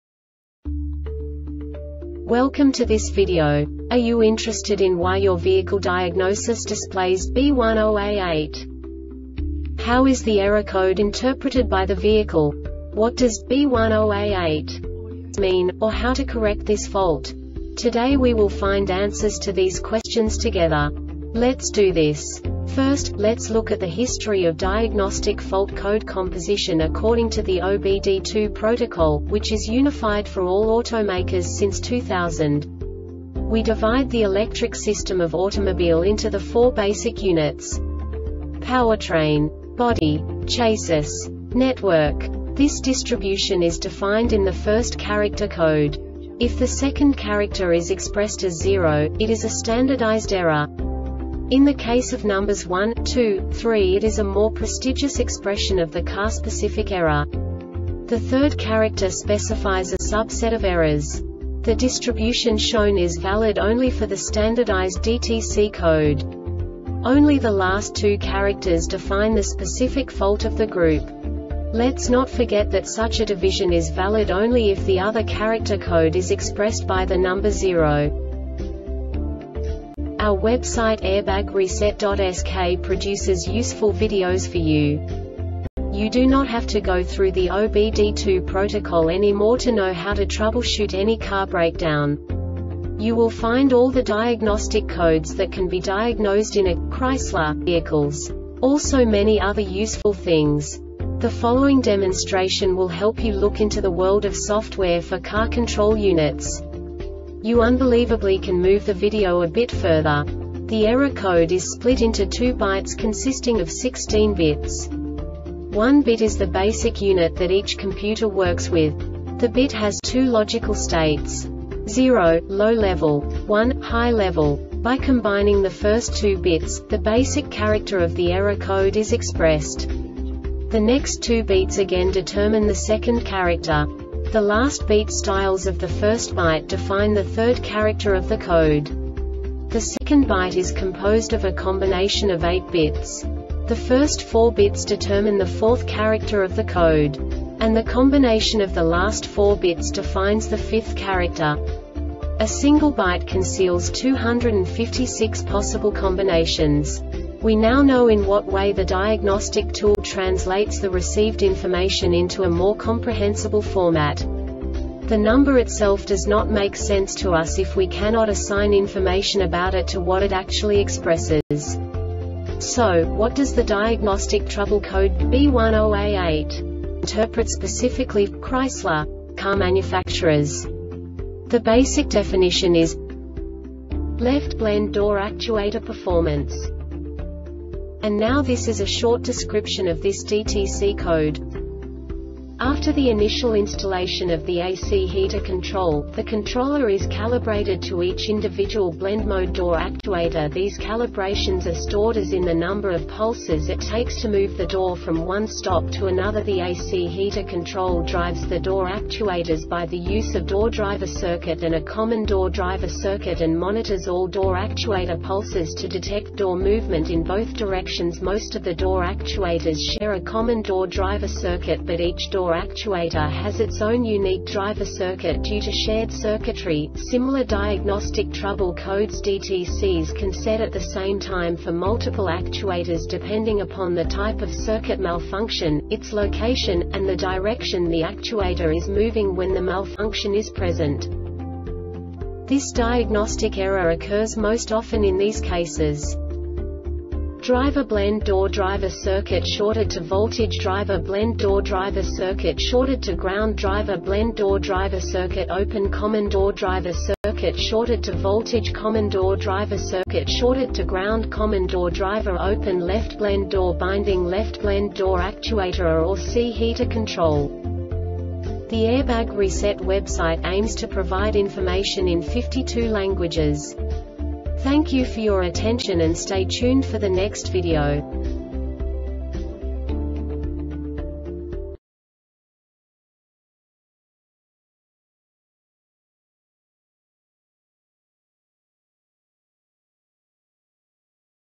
DTC Chrysler B10A8 Short Explanation
Contents:
0:21 Basic DTC analysis according to OBD2 protocol standard.
1:48 Insight into programming
2:58 A diagnostically understandable description of the Diagnostic Trouble Code
3:05 Basic error definition

Make
Chrysler
Code
B10A8
Definition
Left Blend Door Actuator Performance
Description
After the initial installation of the A or C Heater Control, the controller is calibrated to each individual blend or mode door actuator These calibrations are stored as in the number of pulses it takes to move the door from one stop to another The A or C Heater Control drives the Door Actuators by the use of Door Driver circuit and a Common Door Driver circuit and monitors all door actuator pulses to detect door movement in both directions Most of the door actuators share a common door driver circuit but each door actuator has its own unique driver circuit Due to shared circuitry, similar Diagnostic trouble Codes DTCs can set at the same time for multiple actuators depending upon the type of circuit malfunction, its location, and the direction the actuator is moving when the malfunction is present